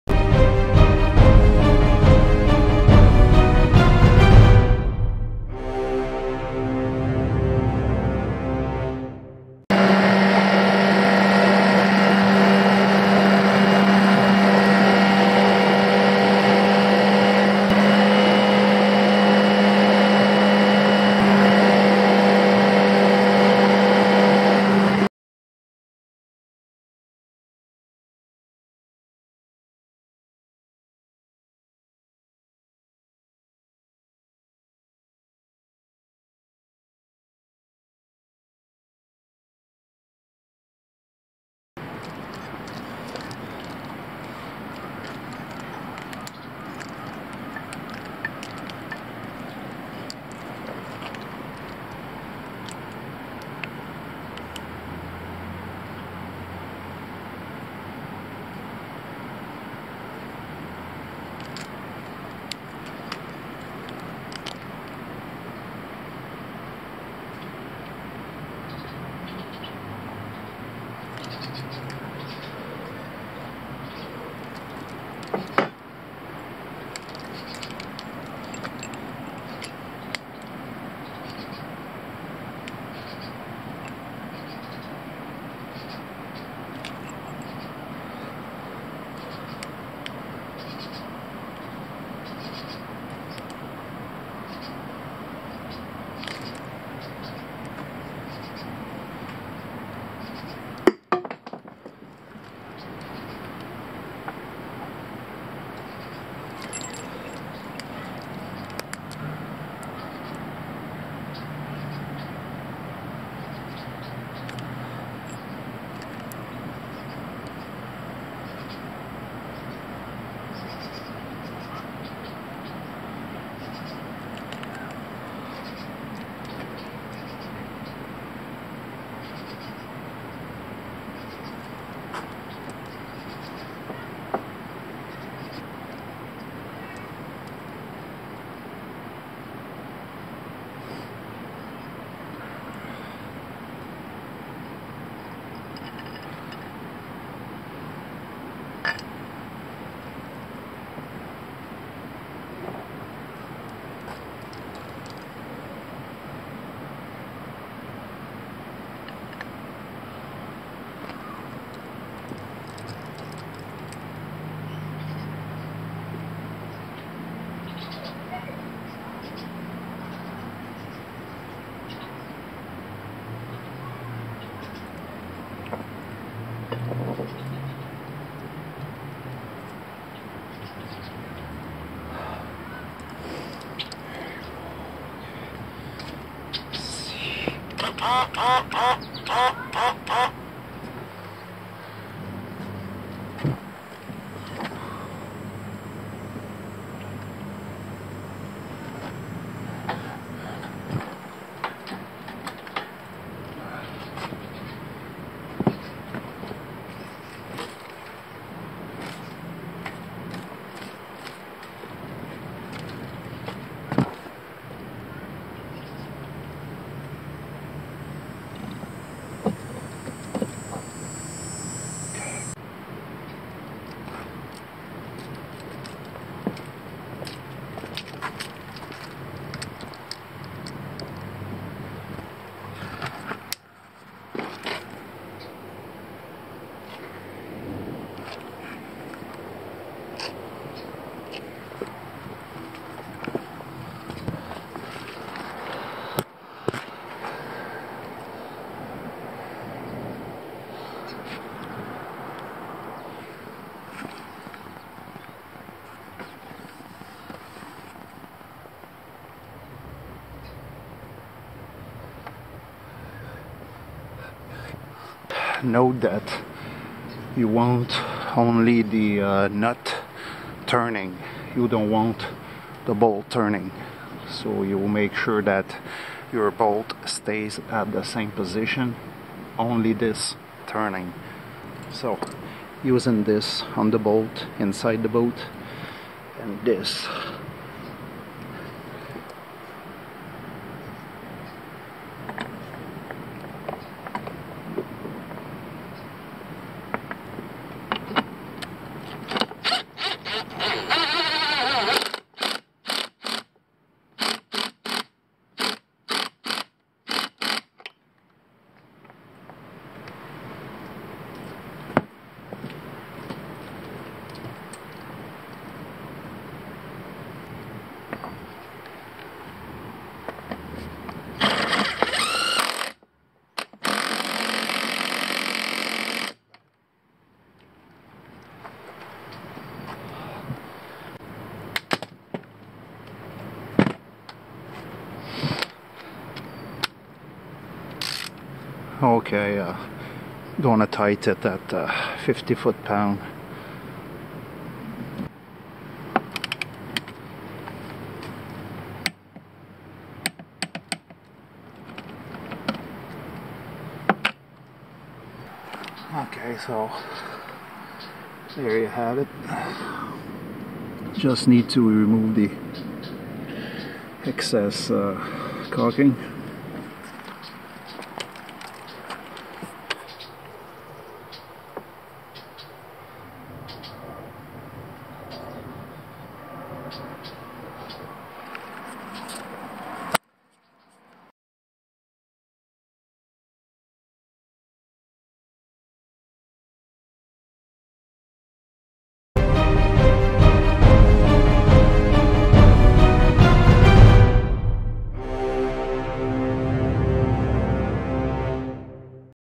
Raising An Outboard Motor Height For Better Performance.Suzuki DF90ATL
Raised my outboard motor for better performance and fuel efficiency.
Task level = easy with engine hoist.
3M Marine Adhesive Sealant Fast Cure 4200
First test dealer installed on the second hole,going up for the third hole !
Stay tune for test result !

Filmed with Olympus TG Tracker in 4K

I am using his music for my Intro and Outro.